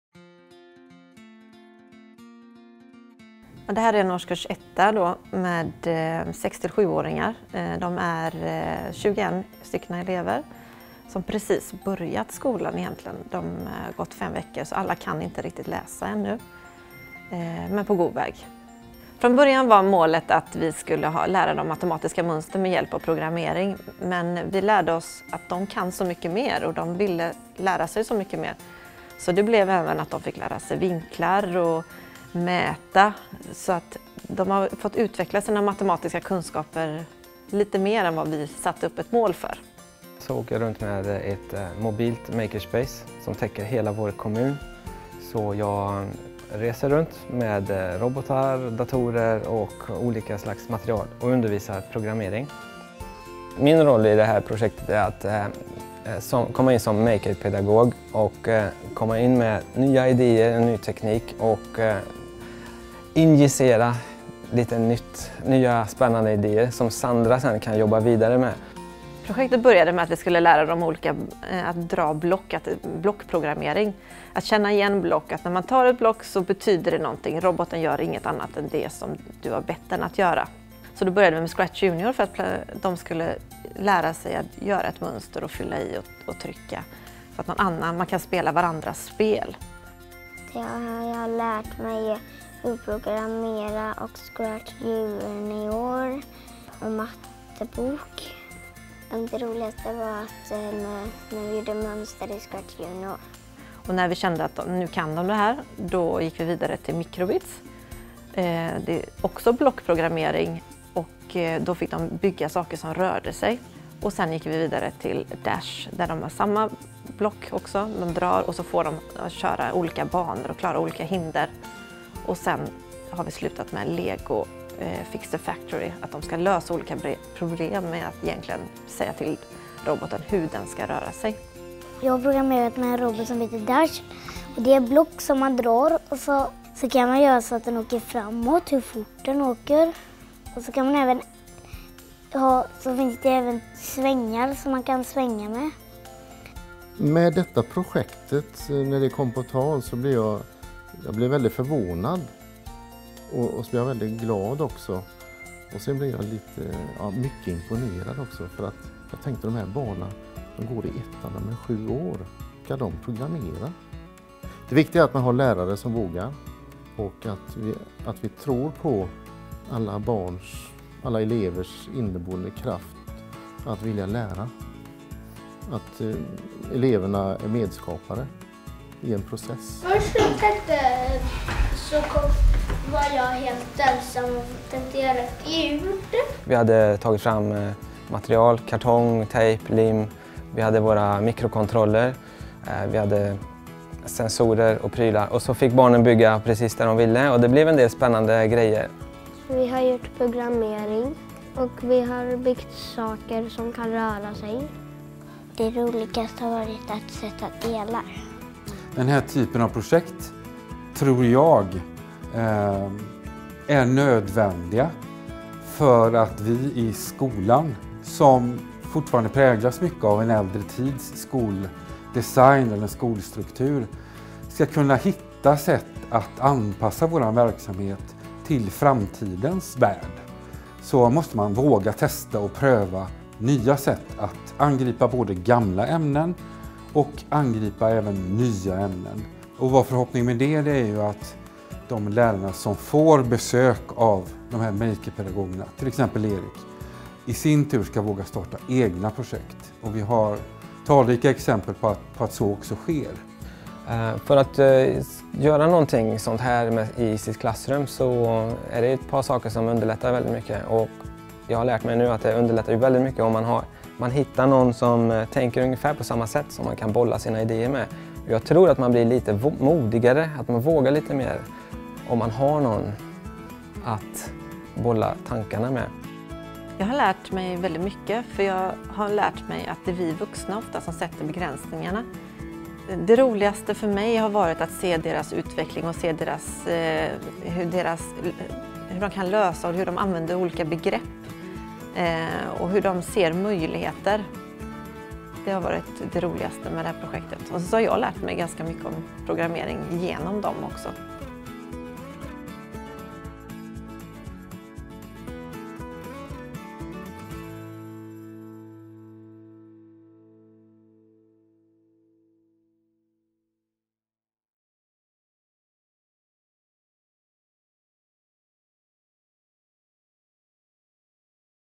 FCL Regio: Mathematical patterns with programming - Öckerö, Sweden
Want to learn more about the project (Agreement number: 2015-1-IT02-KA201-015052)?
This collaborative project in Sweden aimed at combining Mathematics with Technology. Students are collaborating to program and create while understanding and exercising mathematical patterns.